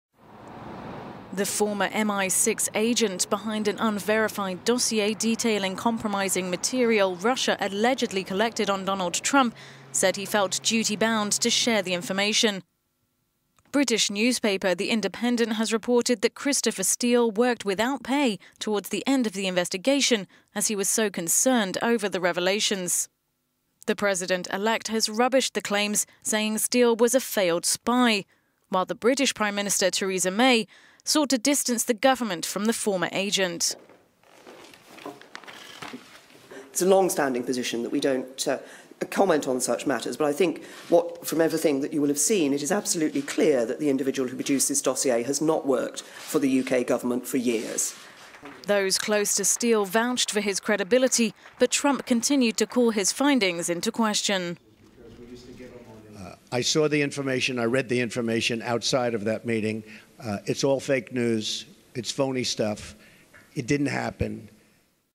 British government distances itself from ex-MI6 agent behind unverified Trump-Russia dossier
The former MI6 agent behind an unverified dossier detailing compromising material Russia allegedly collected on Donald Trump, said he felt 'duty bound' to share the information.

British newspaper The Independent reported that Christopher Steele worked without pay towards the end of the investigation as he was so concerned over the revelations

The president-elect rubbished the claims, saying Steele was a 'failed spy' while the British prime minister Theresa May sought to distance the government…